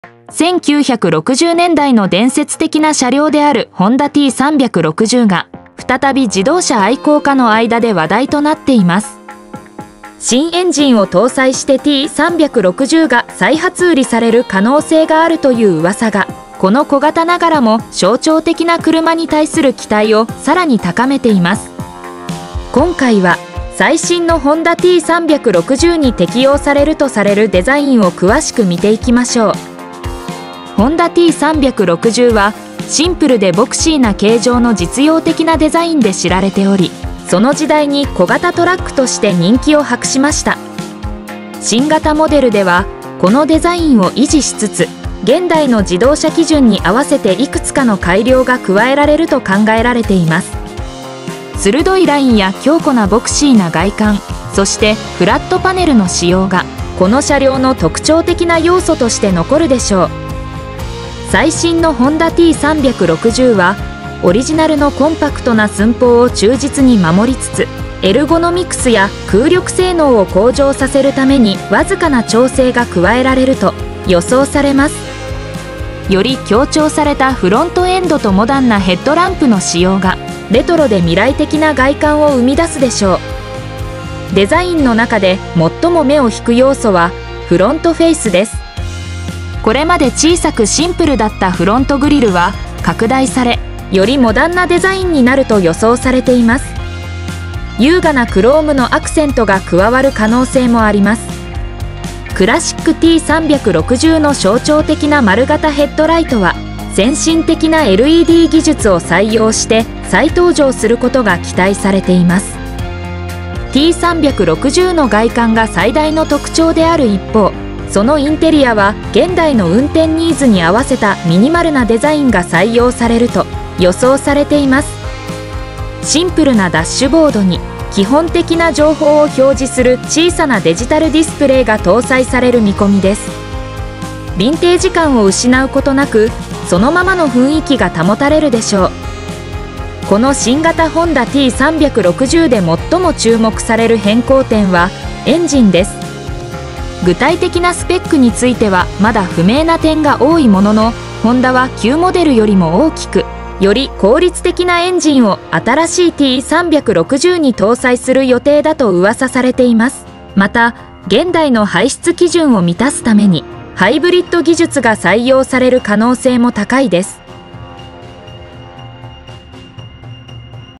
2025年型ホンダT360発表：クラシックな魅力と最先端のテクノロジーが融合
2025年型ホンダT360発表：クラシックな魅力と最先端のテクノロジーが融合

1960年代の伝説的な車両であるホンダT360が、再び自動車愛好家の間で話題となっています。新エンジンを搭載してT360が再発売される可能性があるという噂が、この小型ながらも象徴的な車に対する期待をさらに高めています。今回は、最新のホンダT360に適用されるとされるデザインを詳しく見ていきましょう。

クラシックなデザインに現代のタッチ
ホンダT360は、シンプルでボクシーな形状の実用的なデザインで知られており、その時代に小型トラックとして人気を博しました。新型モデルでは、このデザインを維持しつつ、現代の自動車基準に合わせていくつかの改良が加えられると考えられています。鋭いラインや強固なボクシーな外観、そしてフラットパネルの使用が、この車両の特徴的な要素として残るでしょう。

寸法とプロポーション
最新のホンダT360は、オリジナルのコンパクトな寸法を忠実に守りつつ、エルゴノミクスや空力性能を向上させるためにわずかな調整が加えられると予想されます。より強調されたフロントエンドとモダンなヘッドランプの使用が、レトロで未来的な外観を生み出すでしょう。

フロントフェイスとグリル
デザインの中でもっとも目を引く要素は、フロントフェイスです。これまで小さくシンプルだったフロントグリルは、拡大され、よりモダンなデザインになると予想されています。優雅なクロームのアクセントが加わる可能性もあります。クラシックT360の象徴的な丸型ヘッドライトは、先進的なLED技術を採用して再登場することが期待されています。